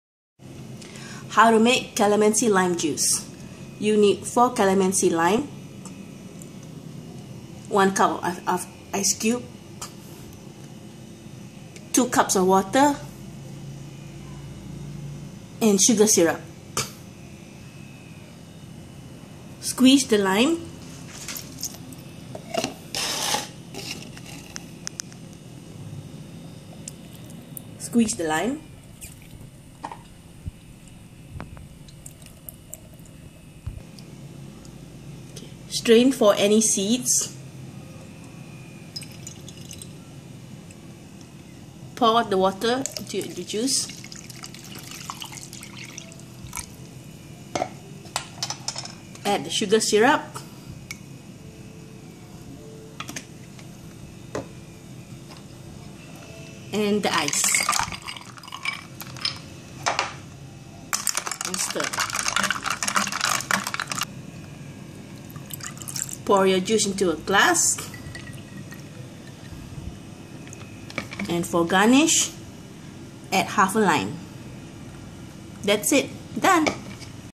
How to make calamansi lime juice
The process of making calamansi lime juice and the ingredients needed.